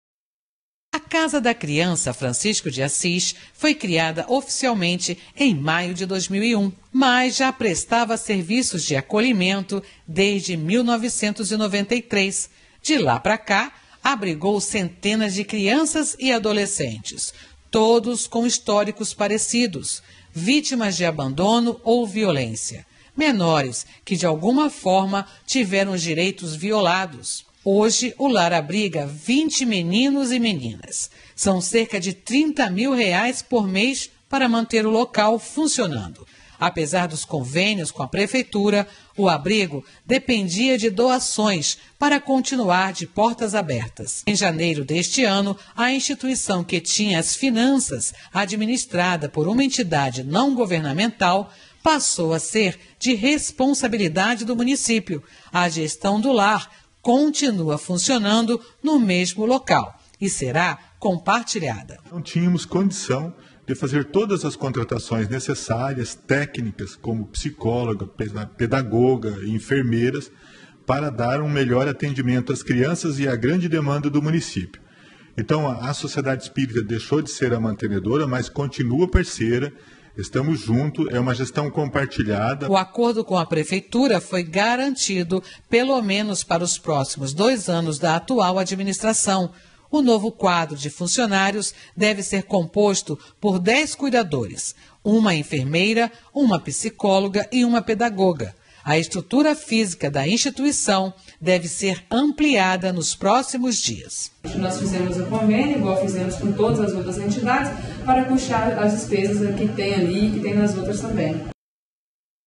Amazônia News l Instituição de crianças e adolescentes é transferido para poder Municipa
Comando de Instituição que abriga crianças e adolescentes é transferido para o Poder Municipal